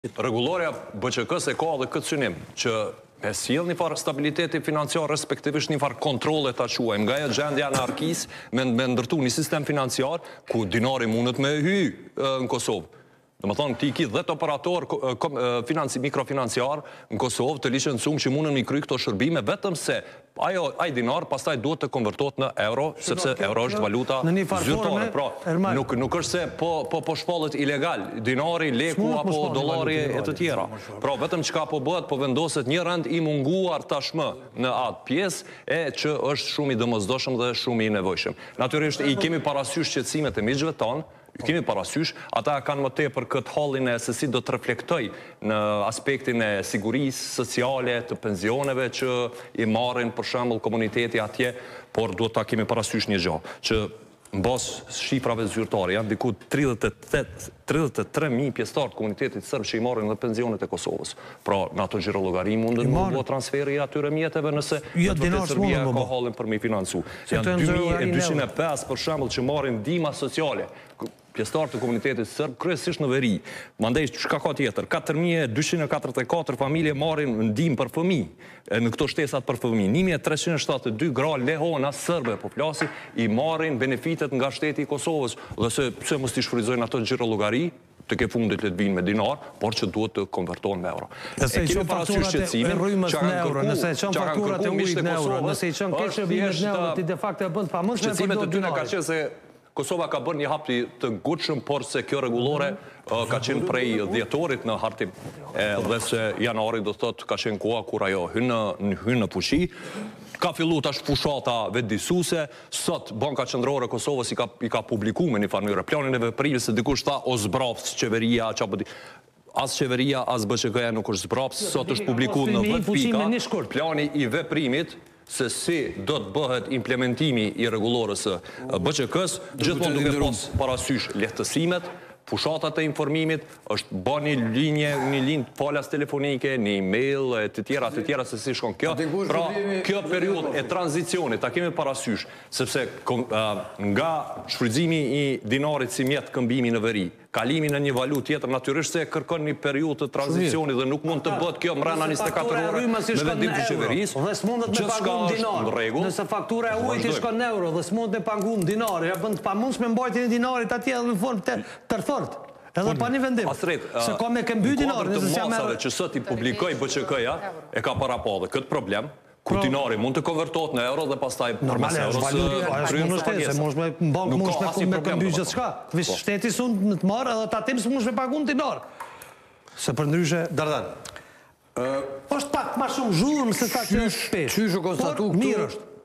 Abrashi: Mijëra serbë i marrin të gjitha benefitet nga Kosova, le t’i shfrytëzojnë ato gjirollogari
“33 mijë serbë marrin pensione edhe në Kosovë” , Artan Abrashi: Mijëra serbë i marrin të gjitha benefitet nga Kosova, le t’i shfrytëzojnë ato gjirollogari